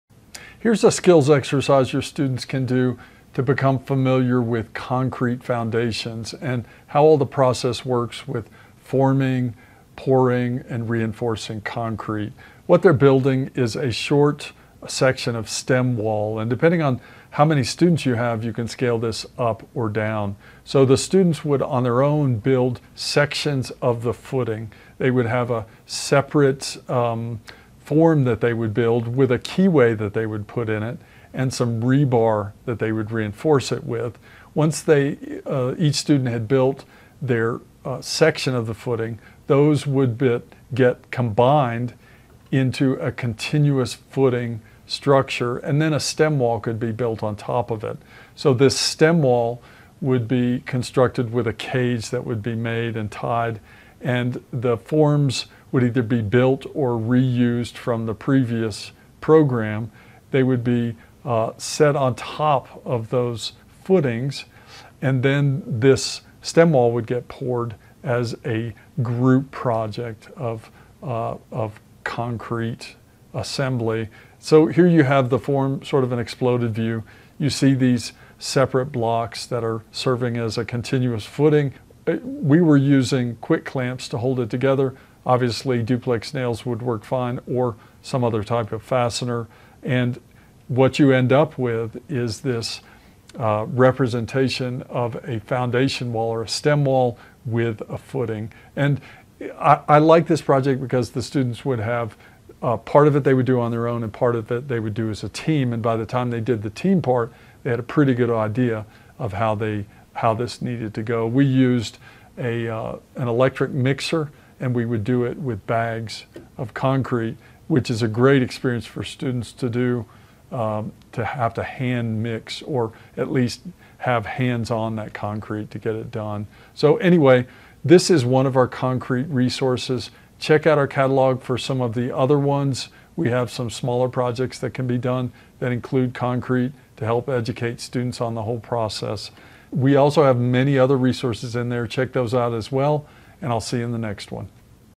Stem wall project - TEACH Construction Training Tip - Construction Training Resources
Another Traning Tip - This one is a build project/skills exercise for concrete lessons - a two-part stem wall build

Some of our Builds and Exercises are available on our website as part of our free resources, while others are only included in our premium subscription.

If you are a premium subscriber all the plan sets are in your resource library.